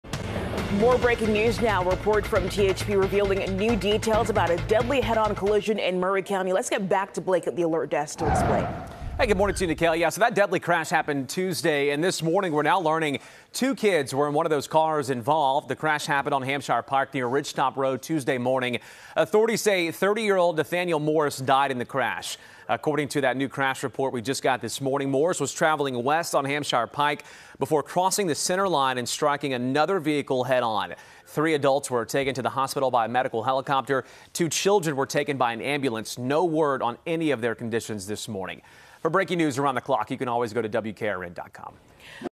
1 dead after head-on crash in Maury County
1 dead after head-on crash in Maury County